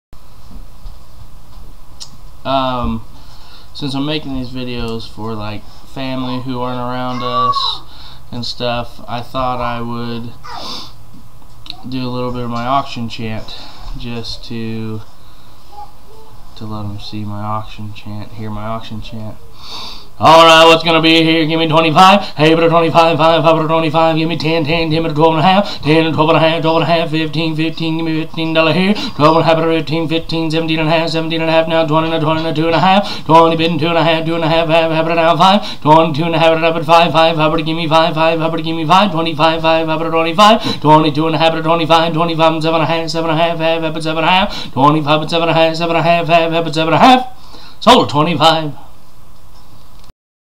My Auction Chant
My family out west wanted to hear my auction chant.  I hope everyone enjoys it! to see my auction website.  You'll have to excuse the wheeze, I had a cold.